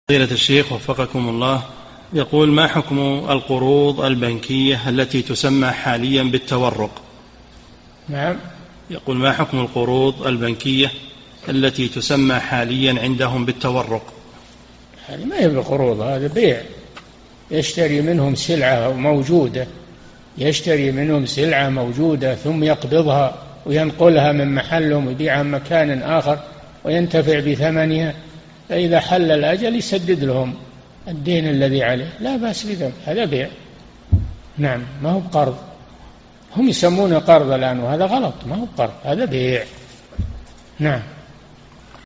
[1709 -3022] ما حكم القروض البنكية التي تسمى حالياً بالتورق؟ - الشيخ صالح الفوزان
-"[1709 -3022] السؤال: ما حكم القروض البنكية التي تسمى حالياً بالتورق؟ نعم.
الجَوَابُ: هذي ما هي بقروض هذي بيع، يشتري منهم سلعة موجودة ثم يقبضها وينقلها من محلهم ويبيعها من مكان أخر، وينتفع بثمنها، فإذا حل الأجل يسدد لهم، الدين الذي عليه لا بأس بذلك هذا بيع، نعم ما هو بقرض، هم يسمونه قرض الآن وهذا غلط ما هو بقرض هذا بيع.
للاشتراك في قناة الشيخ صالح الفوزان

للاستفسار والإقتراحات والدعم
رئيس الفريق
لمشروع كبار العلماء